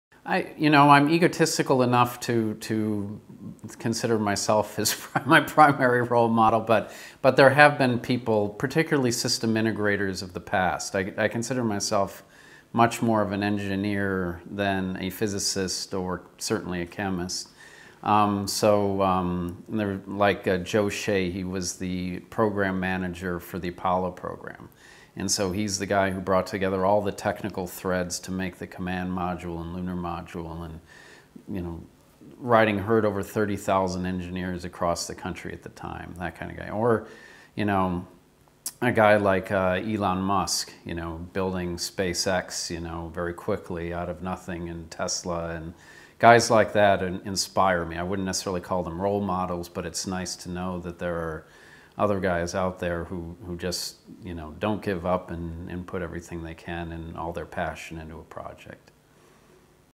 "System integrators of the past." Eric Betzig, 2014 Nobel Prize in Chemistry, on his role models
Eric Betzig, 2014 Nobel Prize in Chemistry laureate, was interviewed on 6 December, 2014 at the Nobel Museum in Stockholm, Sweden.

The Nobel Prize in Chemistry 2014 was awarded jointly to Eric Betzig, Stefan W. Hell and William E. Moerner "for the development of super-resolved fluorescence microscopy".